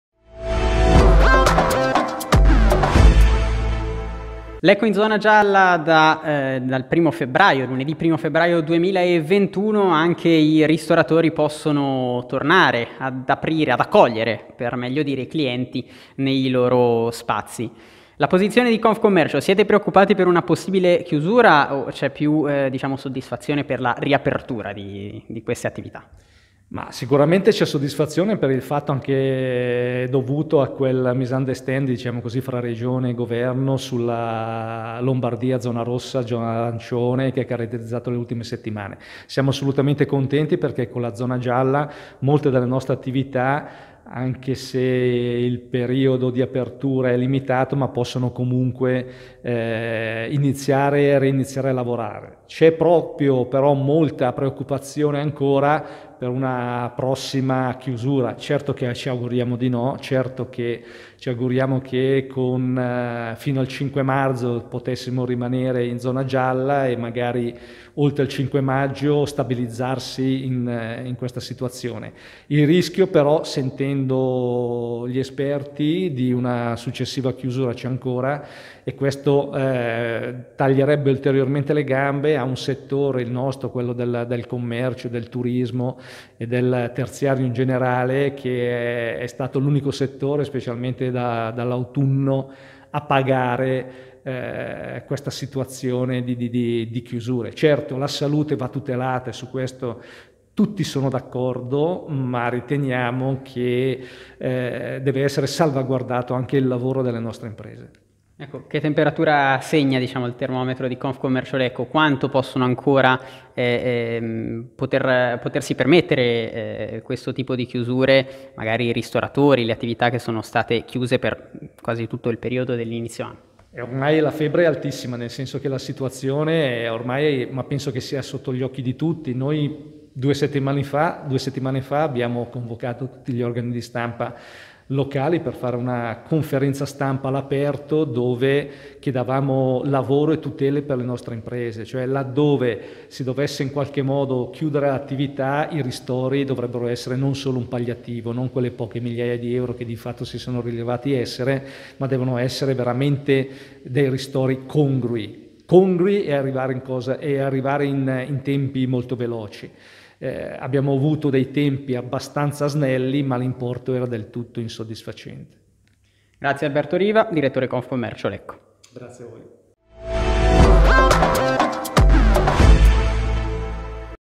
ZONA GIALLA: LA SITUAZIONE DEI COMMERCIANTI A LECCO
Soddisfazione per la zona gialla o preoccupazione per altre possibili chiusure? La parola a Confcommercio Lecco